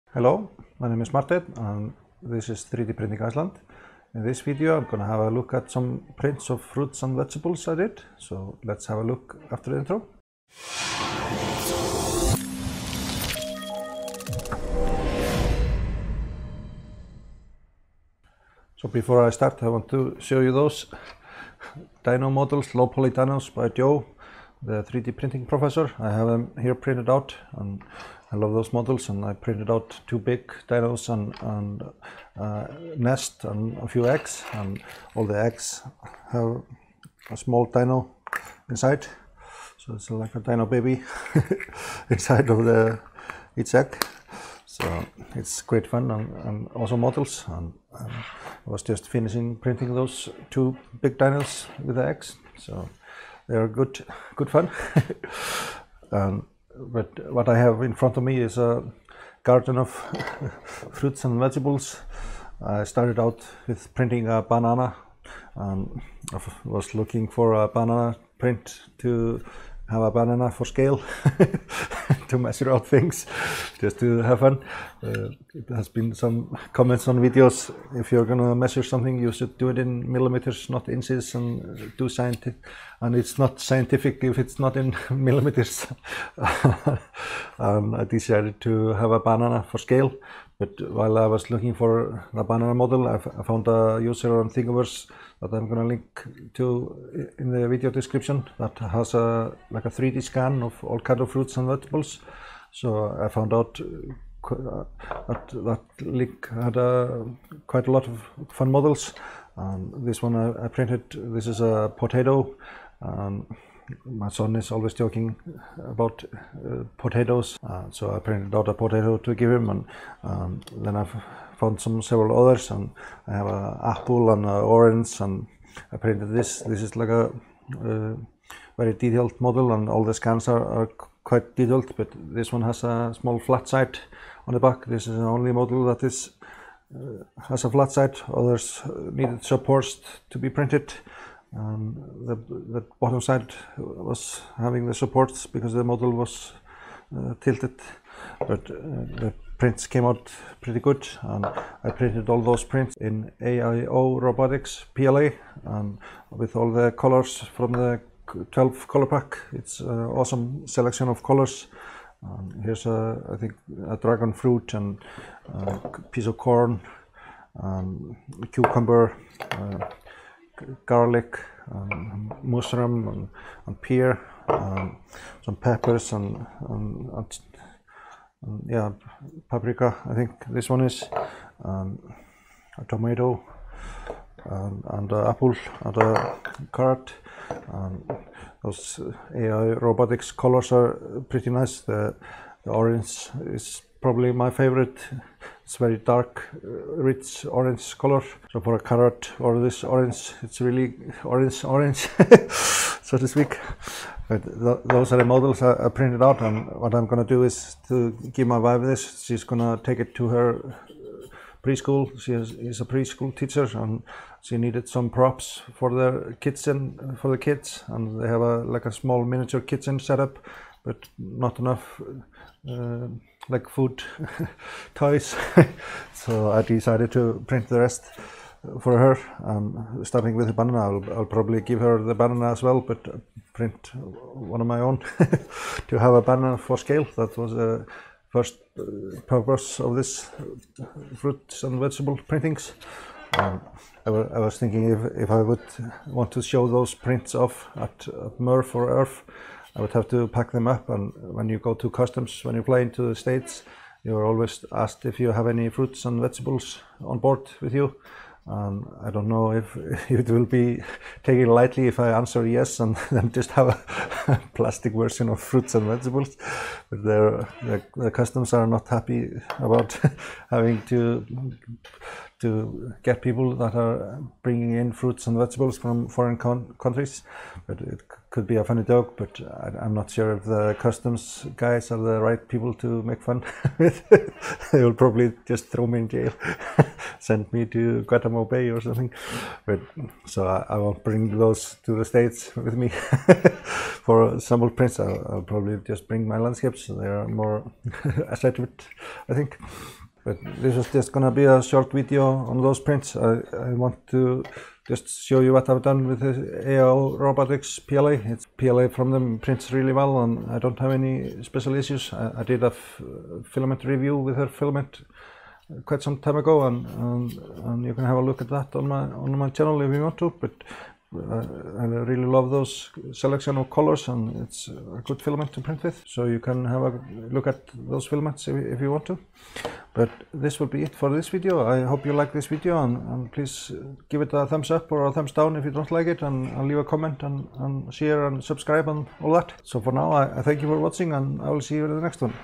3D Printed Fruits and Vegetables
In this video I show you a set of PLA prints of some fruits and stuff.